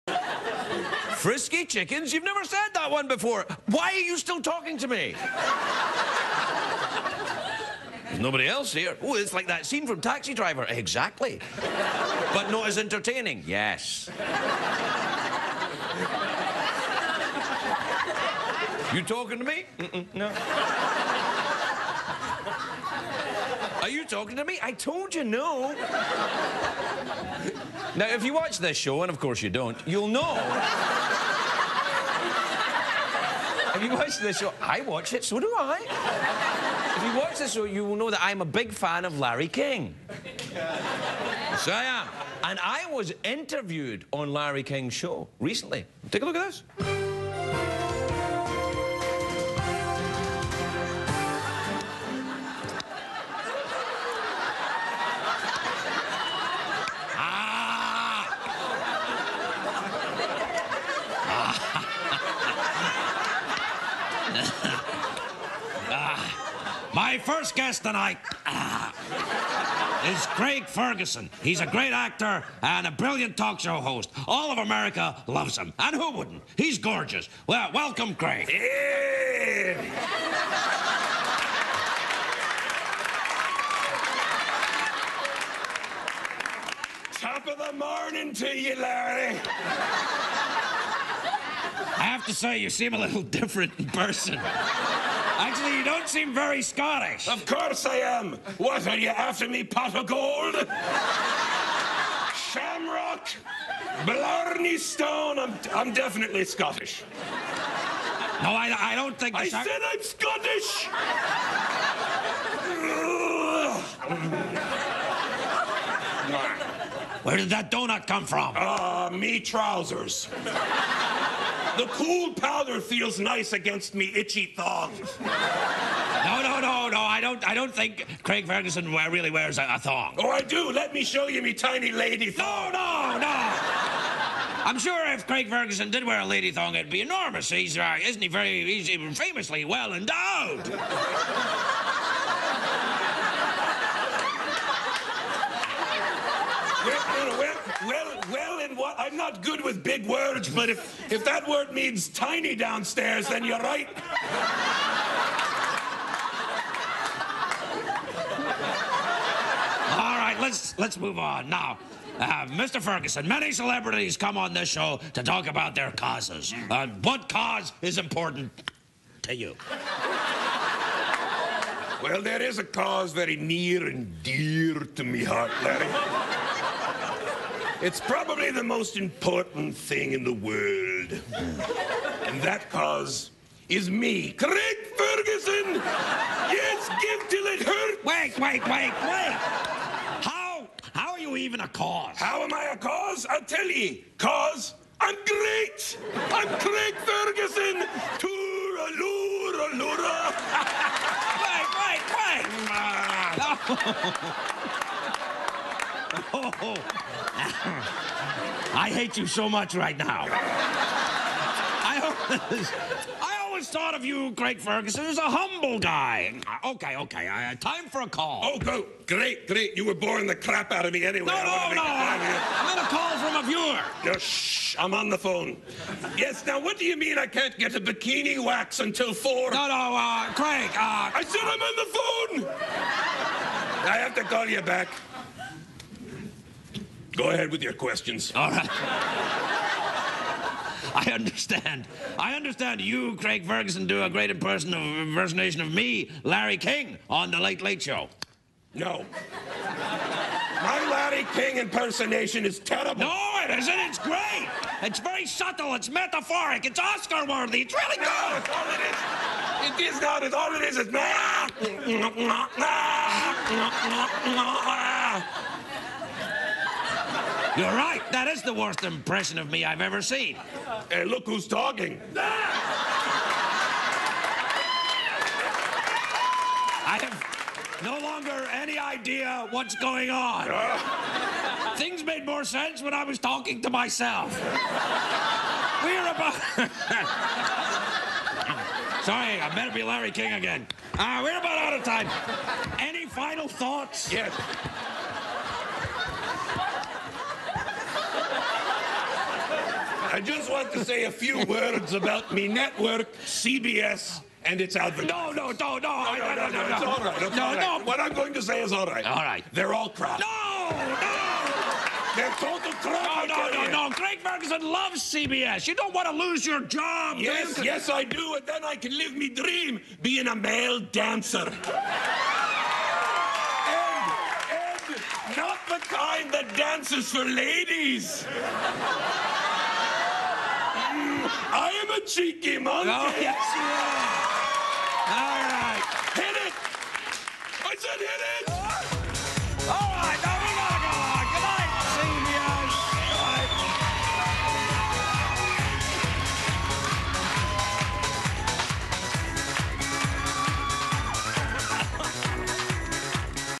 "Larry" Interviews "Craig Ferguson"
The real Craig (as Larry King) interviews Fake Craig.  The hay stack hair, freckle-masking/clearly delineated orange makeup, bad Scottish accent, sloppy, voracious eating habits, crude, loud, short-tempered and egotistical behavior - John Reynolds as Fake Craig misses no opportunity to caricature his boss, leaving poor Craig helpless, irate and cracking up! January 29, 2008

Footnote: In the intro, Craig is talking to his original sidekick, the invisible Davis. :D